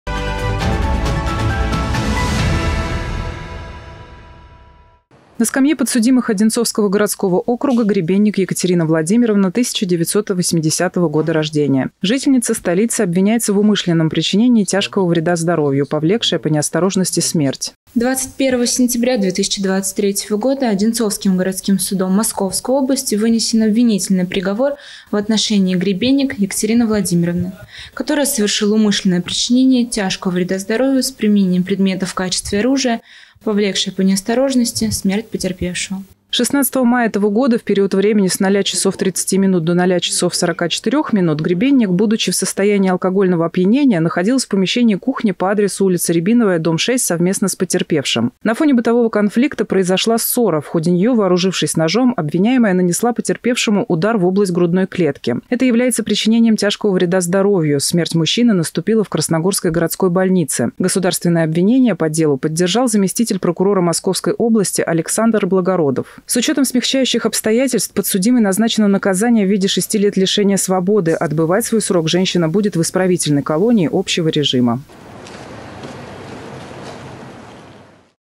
Заседание суда
Семейная ссора привела Екатерину ГребеннИк на скамью подсудимых. Заседание по уголовному делу прошло в городском суде.